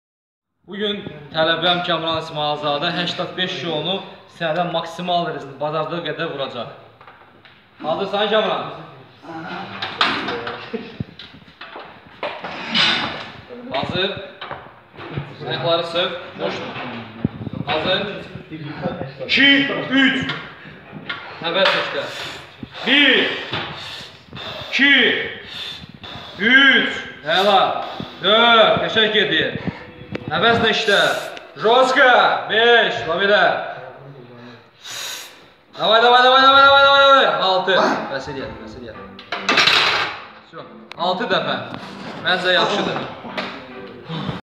Ateks Motivator və tələbəsi Kamran İsmayılzadə / öz çəkisi 67 kg / Sinədən qaldırma 85 kg x 6 say
Break your limits ! - Преодолей свои пределы !
Road to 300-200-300 ! - Путь к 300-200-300 !
Without straps - Без лямок
Without wraps - Без бинтов
Without belt - Без ремня
Without steroids - Без стероидов